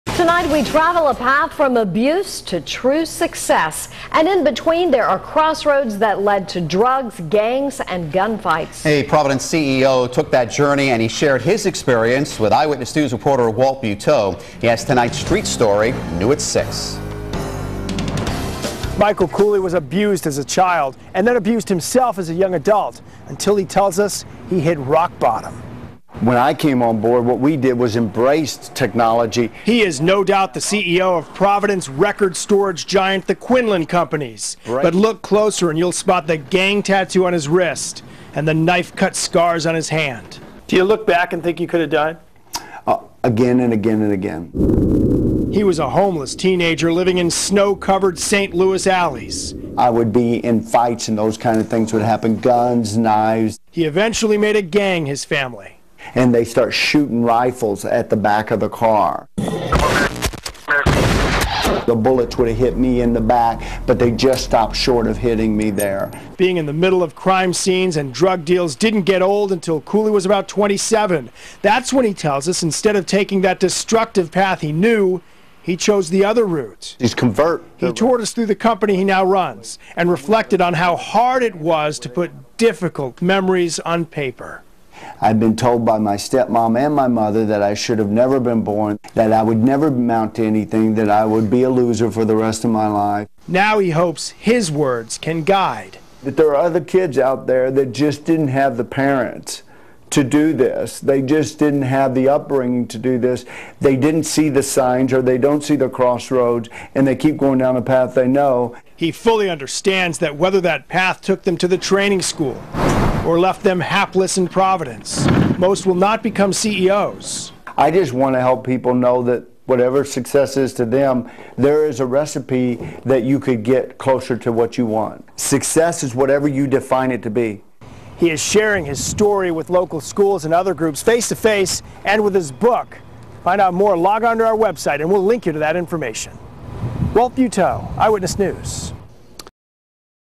Michael Cooley - WPRI, Providence, RI
When Michael Cooley returned to his childhood home in St. Louis, many of his old friends offered him this greeting: 'Cooley, I thought you were dead.' And by many accounts, he should have been dead.

Michael Cooley was a member of two dysfunctional families. His mother in St. Louis was an alcoholic. His stepmother and stepbrothers in Gainesville, Texas, abused him relentlessly—both physically and verbally. As a child, Mike bounced from school to school and from city to city. As a teenager, he turned to gangs and drugs for a sense of acceptance. Eventually, he ended up homeless, sleeping on rooftops and in alleys, and living out of his car.

But after living as an abused child, a homeless and drug-addicted teenager, and a completely lost young adult, Mike decided to start over as a different person. Rock Bottom—From the Streets to Success tells Mike's story from his childhood in the 1960s until today. Mike escapes the darkness of his past and eventually puts his life back together to become a success in business and in his personal life. Follow Mike as he overcomes the hardships of his childhood and shares his experiences in hopes of helping others who may be facing their own challenges.